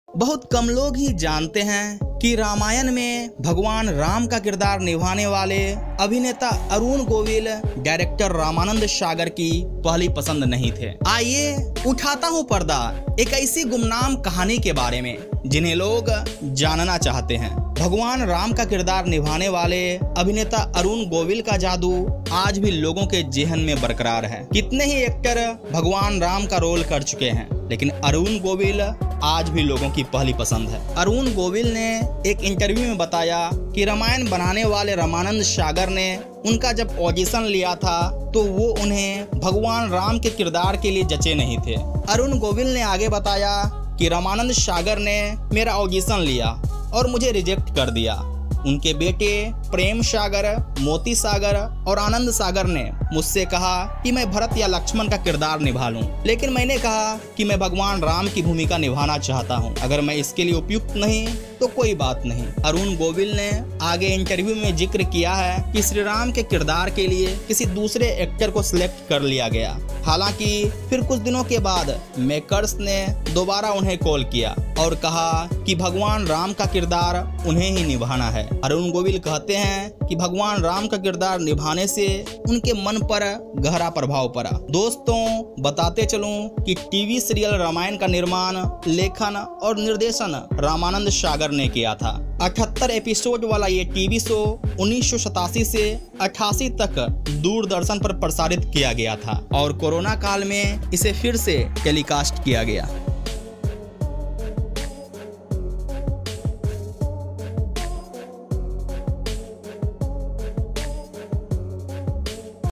रामानंद सागर को अरुण गोविल पसंद नहीं थे, फिर क्यों उसने रामायण में उसे श्रीराम बनाया ?
गुमनाम नायक | Gumnaam Nayak | Unsung Heroes | Ramayan | Arun Govil | Ramanand Sagar |

आदरणीय दोस्तों 🙏